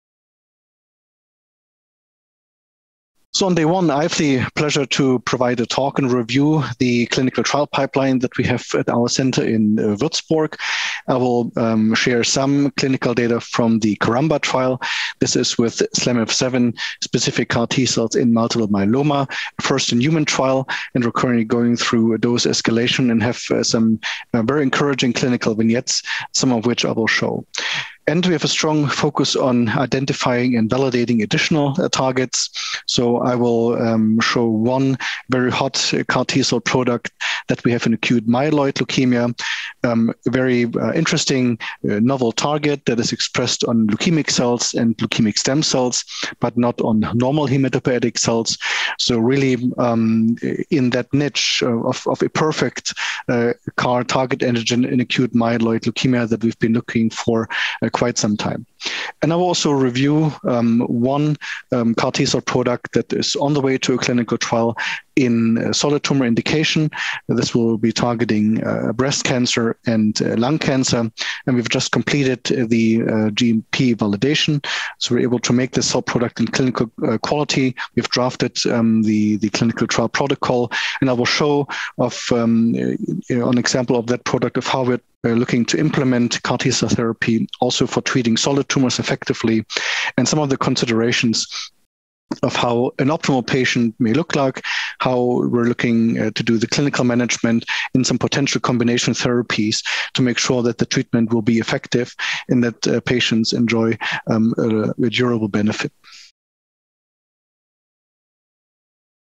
CAR-T trials and research at UKW
Michael Hudecek, MD, University of Würzburg, Würzburg, Germany, gives an update of the chimeric antigen receptor T-cell (CAR-T) clinical trials and research conducted at the University Hospital of Würzburg (UKW). Dr Hudecek talks on the ongoing CARAMBA-1 trial evaluating autologous SLAMF7 CAR-Ts in multiple myeloma (NCT04499339), and comments on a novel promising target antigen for CAR-T therapy in acute myeloid leukemia (AML). He also explains that a clinical trial for a CAR-T product for breast cancer and lung cancer will be launched soon. This interview took place at the EBMT-EHA 4th European CAR T-cell Meeting in which was held virtually in 2022.